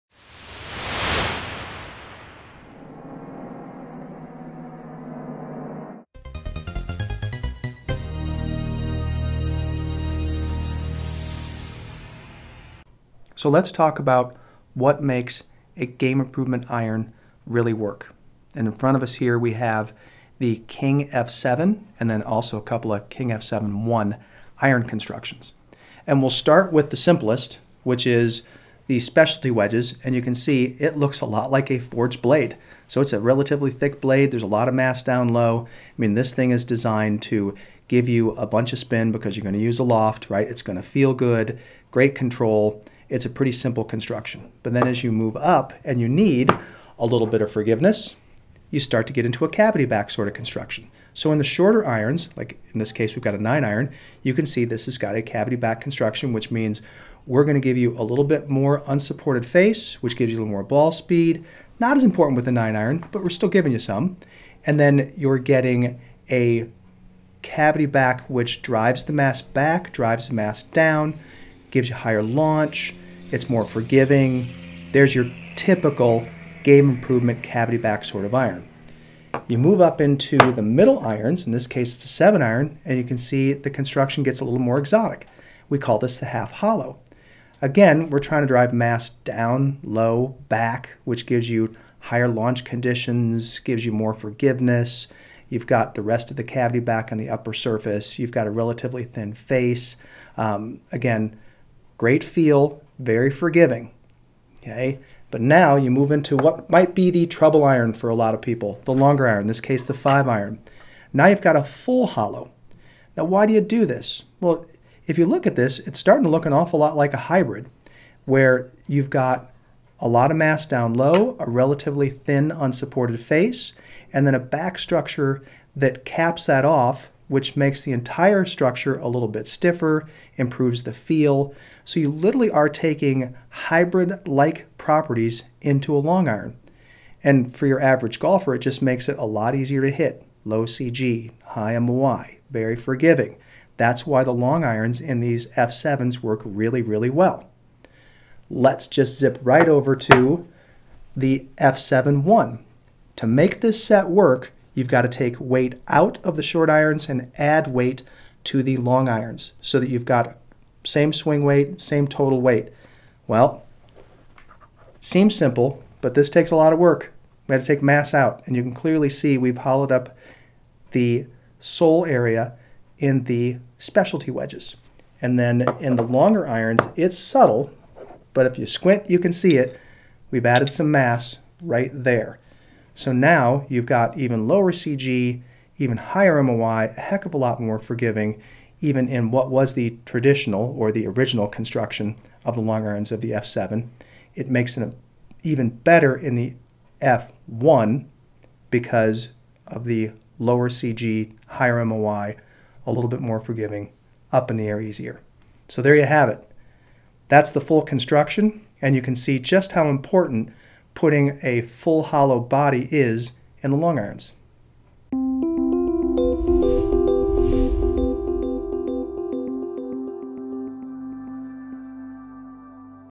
What Makes Golf Clubs Forgiving?
You have all heard the terms and buzz words associated with iron types and most when you look at the different sets lined up, you can see the larger vs smaller club heads, technology in the cavity and so much more packed into irons today. But have you ever asked yourself the question what makes Game Improvement irons forgiving? What design qualities do different irons have that would make the ball react differently?

THP TV sat down with Mike Yagley, Director of Innovation and Testing at Cobra Golf, and had him show the internal structure of different irons and what they are designed to do. From muscle back to completely hollow body designs, the explanations of the design role in forgiveness and more is discussed.

The places to find us for tons of contests and events.